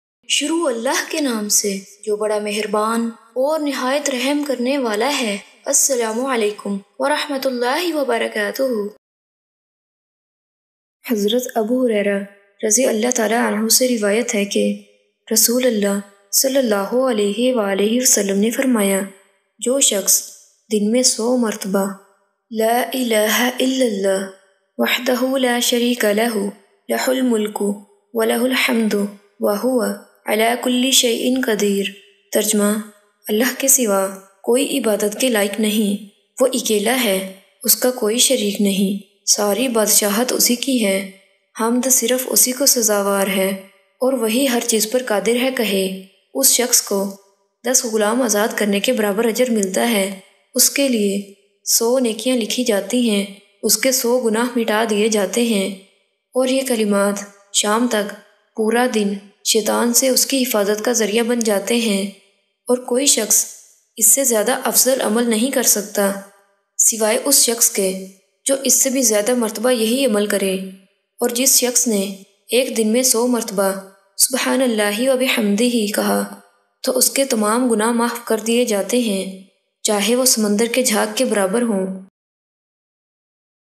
Beautiful Hadith In Urdu ||Hadees Shareef ||Hadees E Nabviﷺ ||Nabiﷺ Ki Batein ||Bareerah Voice
ASSLAM O ALAIKUM and welcome to Bareerah Voice channel. Sweet viwers today i will tell you a beautiful hadees shareef from Miskat shareef Hazrat narrated that prophet Muhammadﷺ said.